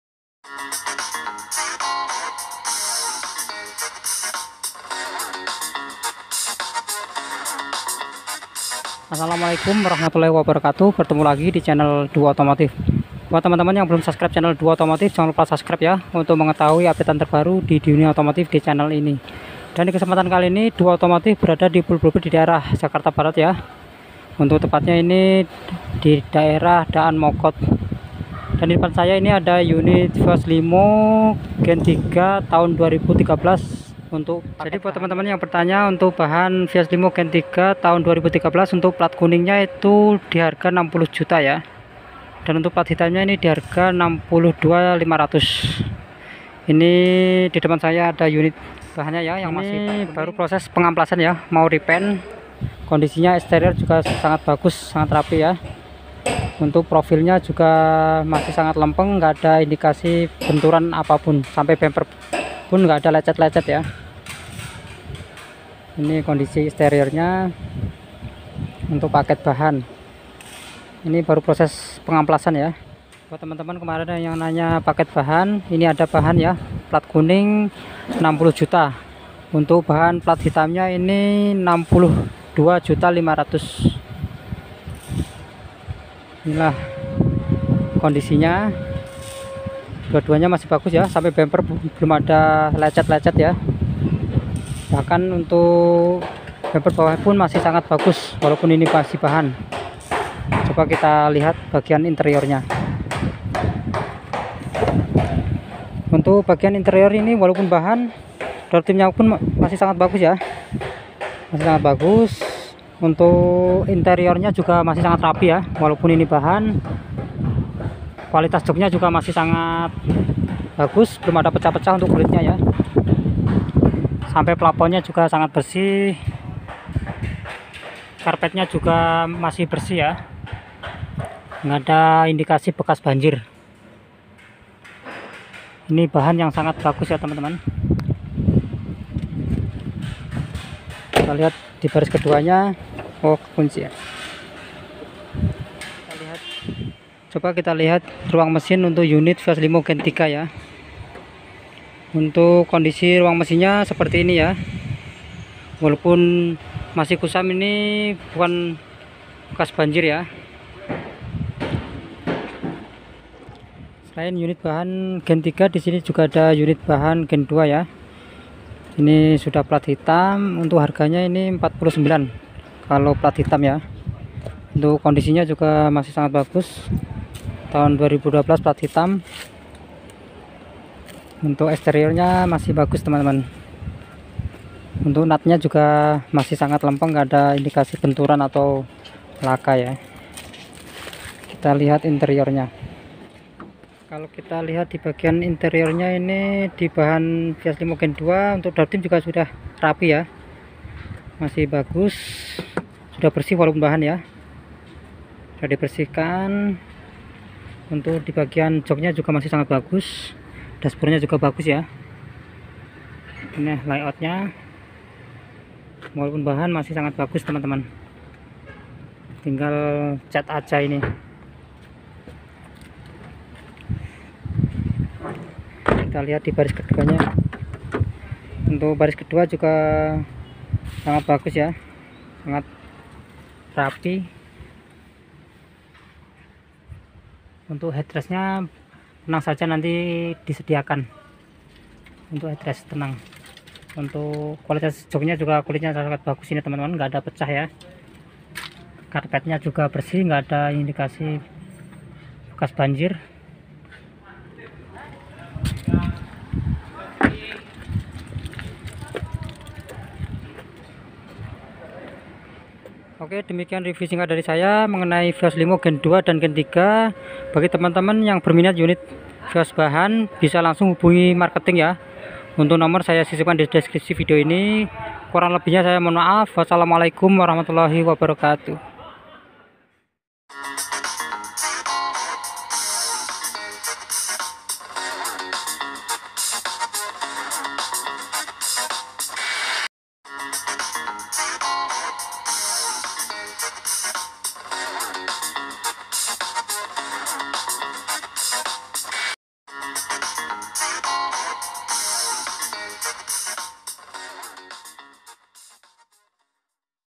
Bahan Vios Limo Gen 3 Bekas Taxi Bluebird
Di Publikasikan Tanggal 7 Desember 2020
Hallo semuanya kali ini saya akan mengajak kalian semua melihat stok bahan vios lino gen 3 bekas taksi bluebird.